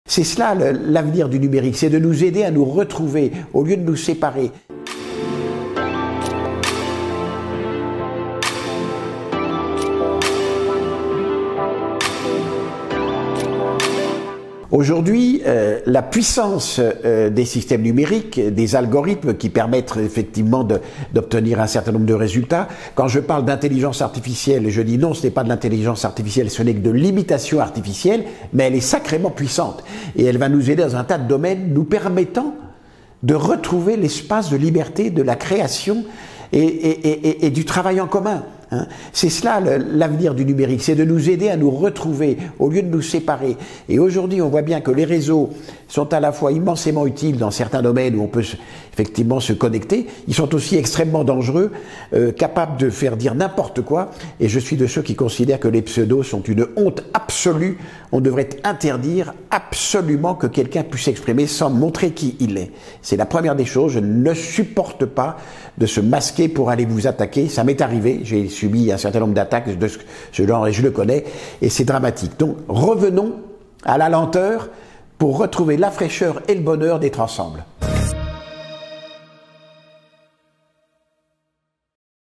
Temps, algorithmes, liberté… De quoi nous interroger ?
Episode 14 de la série Social Book avec Guy Vallancien pour la publication de « A la recherche du temps perçu » paru via Librinova.